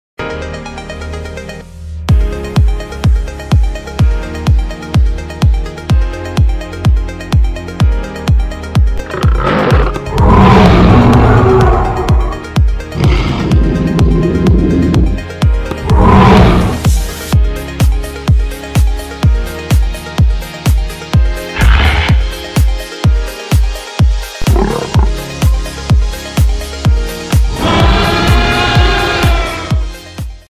Scary T-Rex Visible Legs Costume | Dinosaur Costume
Only Dinosaurs' scary T-Rex costume is a unique gift idea for dinosaur lovers. This costume is crafted from the finest material, which will last for years. It is carefully packed and shipped to make sure the dinosaur costume reaches you in perfect condition.